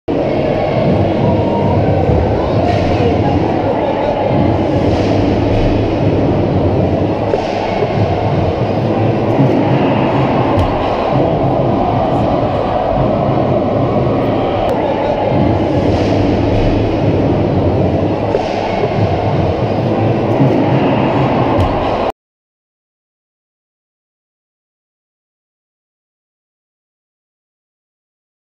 Milton Keynes Lightning 1-4 Guildford Flames 01-10-2016 Flames 3rd
Goal scored by Maslonka with assists from Duggan and Kristoffersson